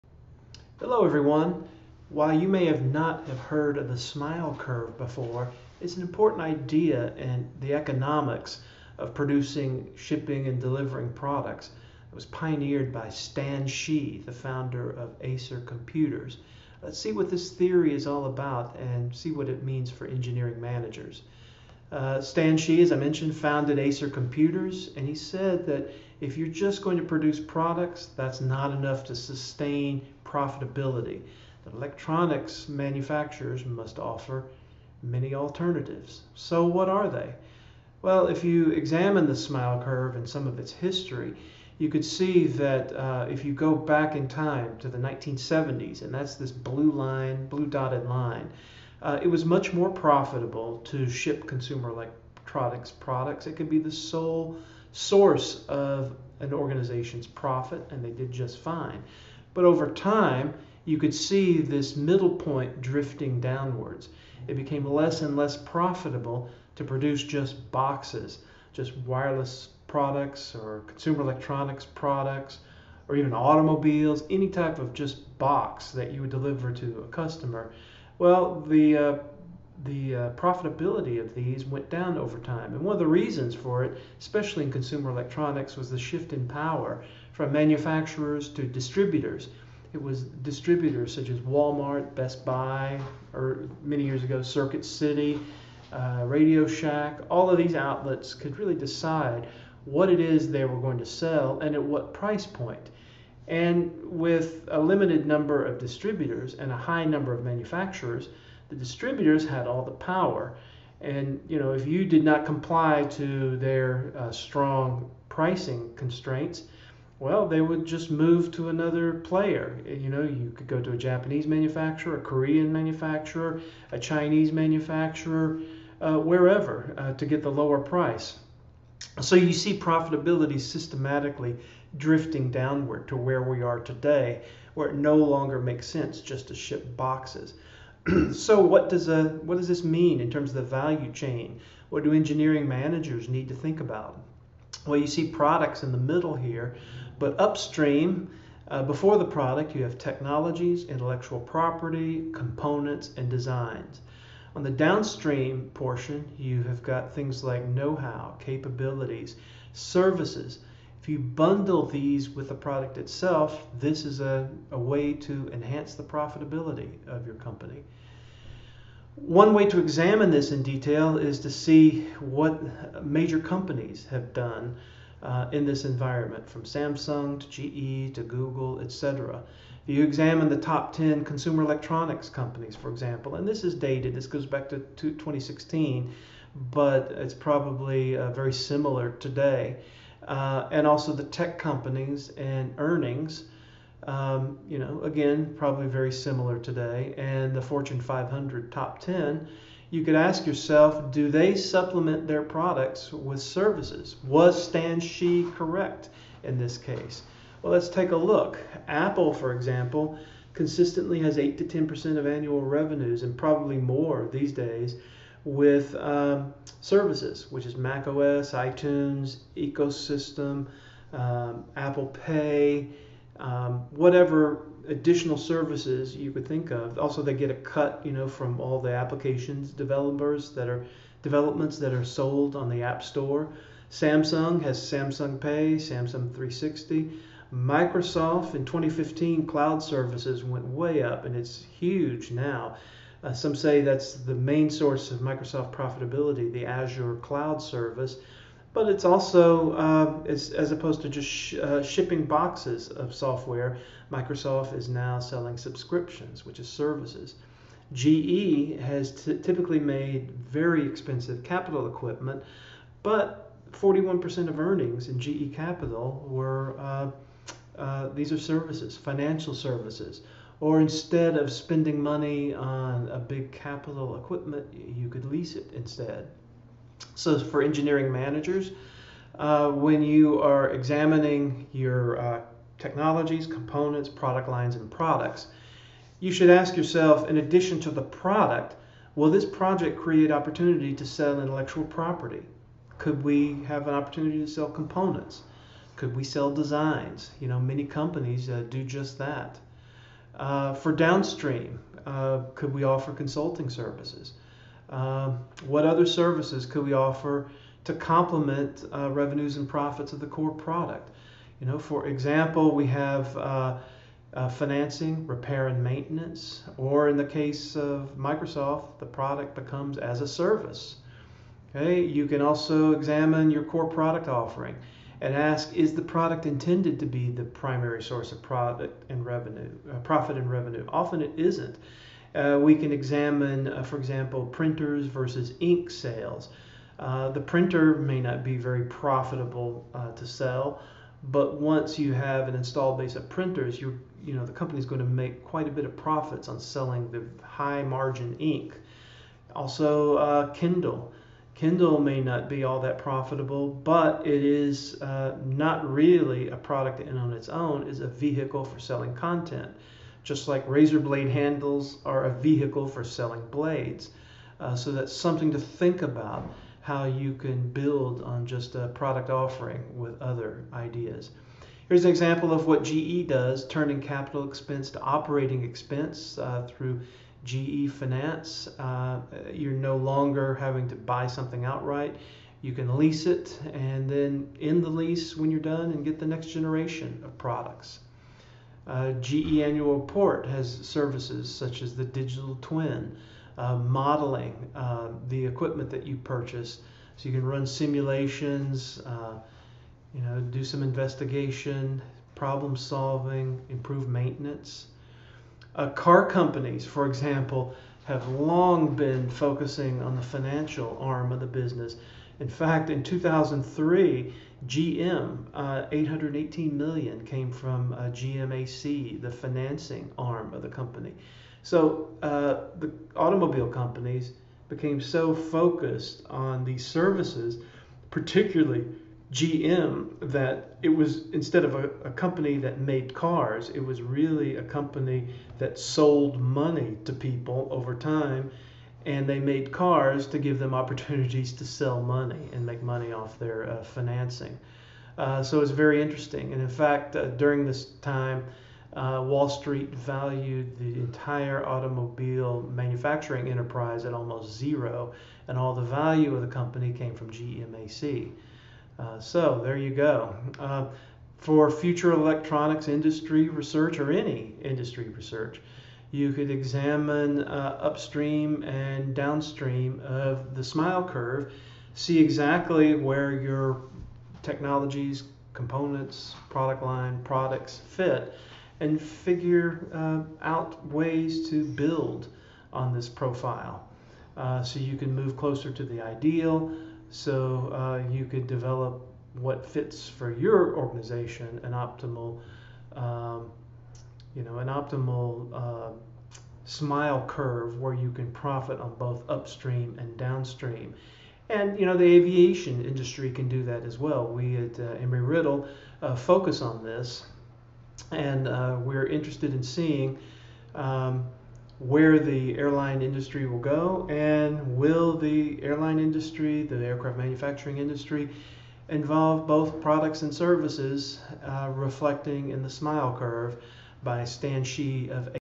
Smile Curve
The Smile Curve and what it means for engineering managers